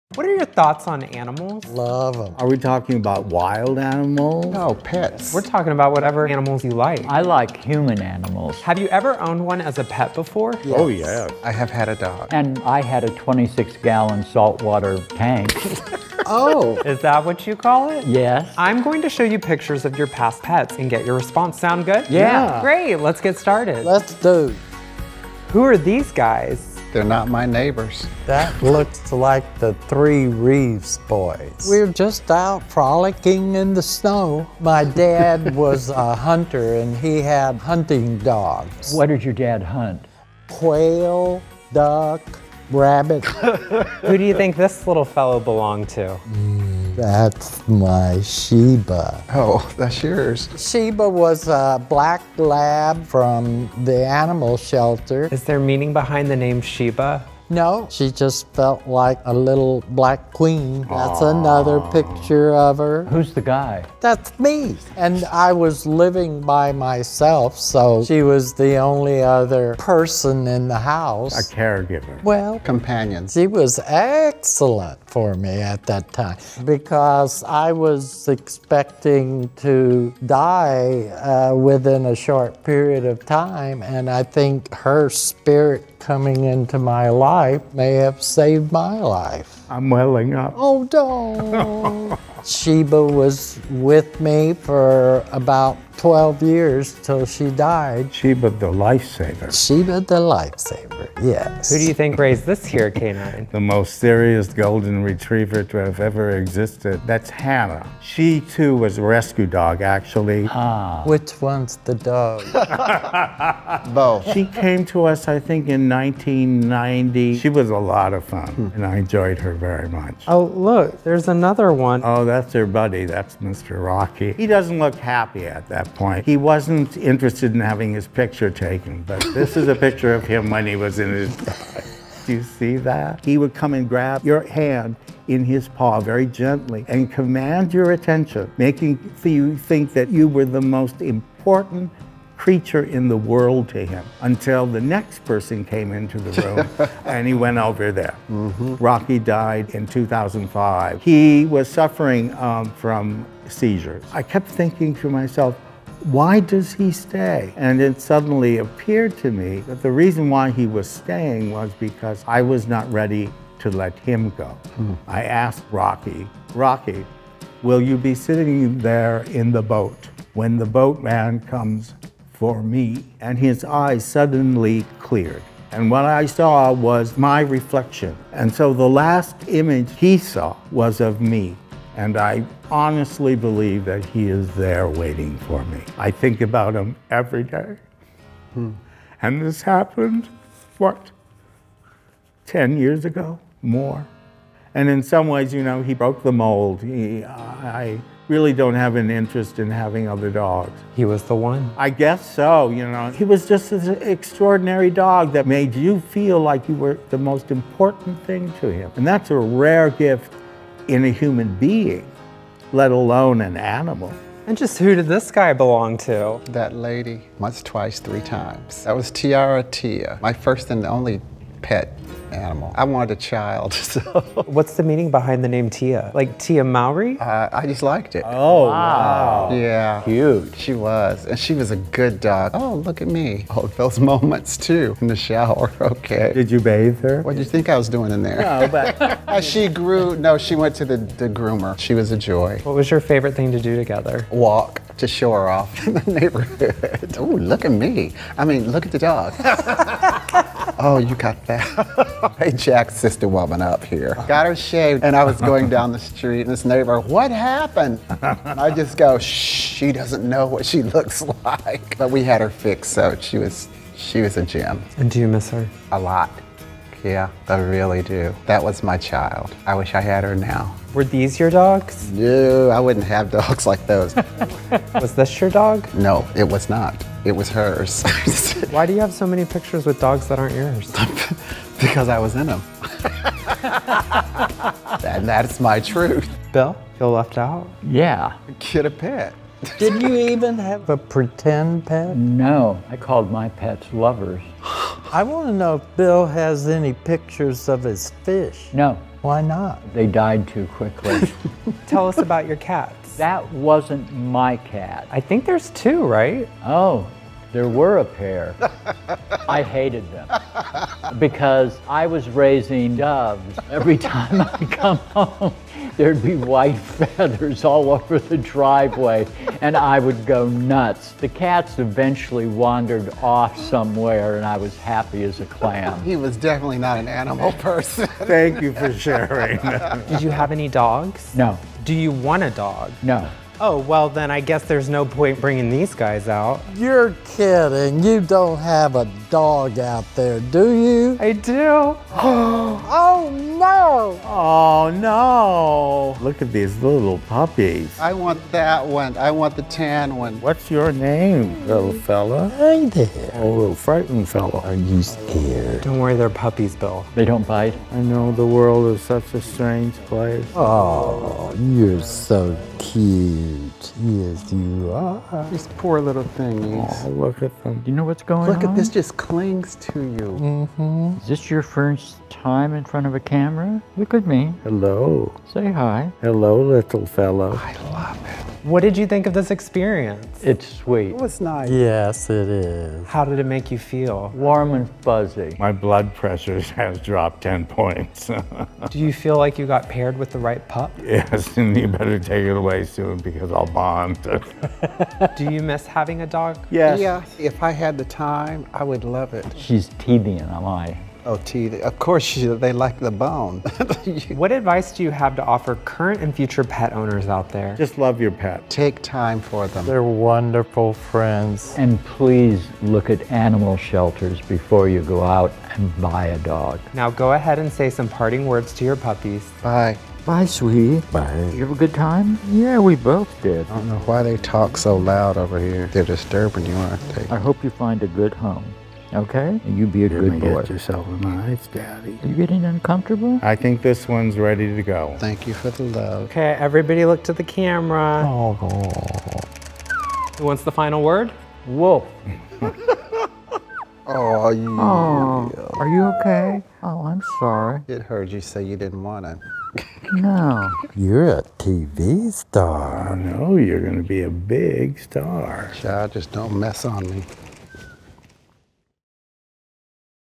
Old Gays Get Surprised With Puppies
"I called my pets lovers."

Have Grindr? Try Xtra For Free

Featuring:
Robert E. Reeves
Bill Lyons

Senior Producer & Editor: Ryan Yezak
Director of Photography: Merlin Showalter
Camera Operator: Daniel Riser
Sound: Nathan Alef
Coordinator: Paige Showalter